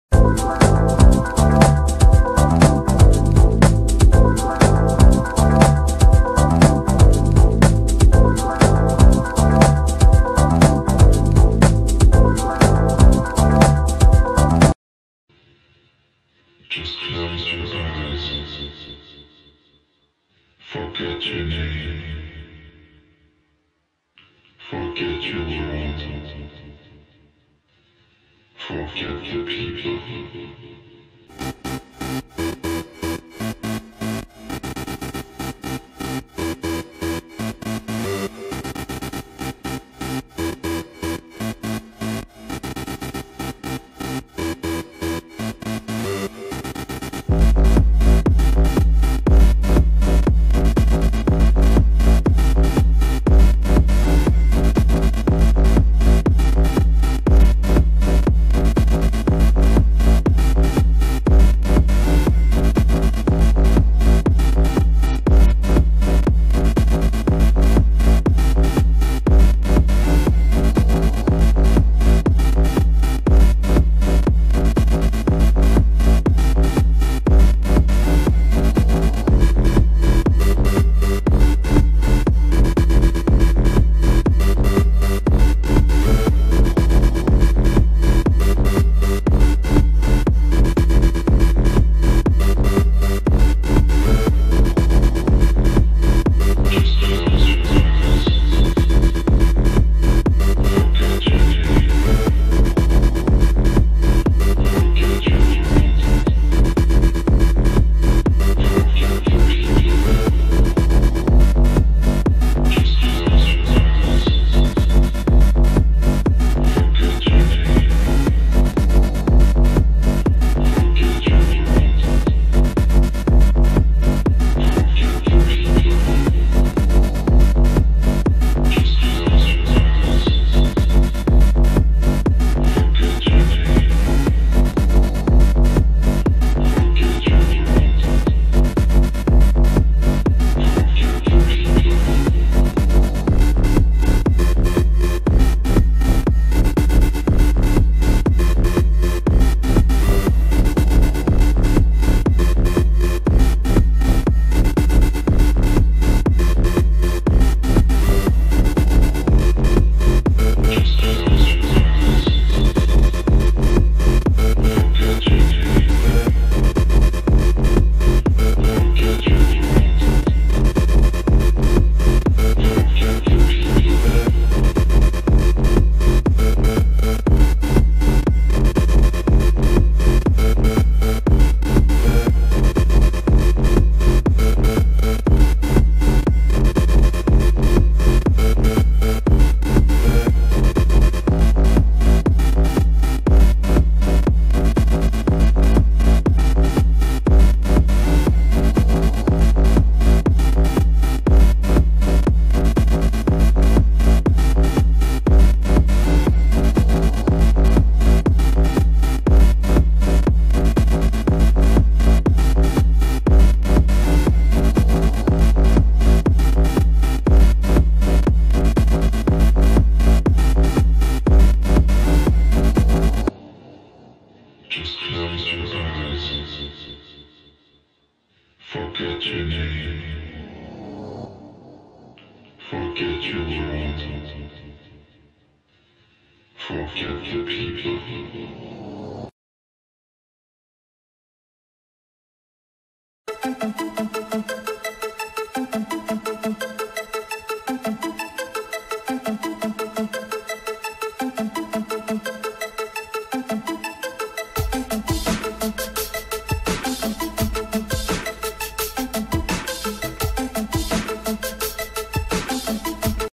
Sprite Slideshow #29: Geodude and Evolutions!
Sprite Slideshow #29! It's Geodude, Graveler, and Golem!

(HUGE Thanks to them)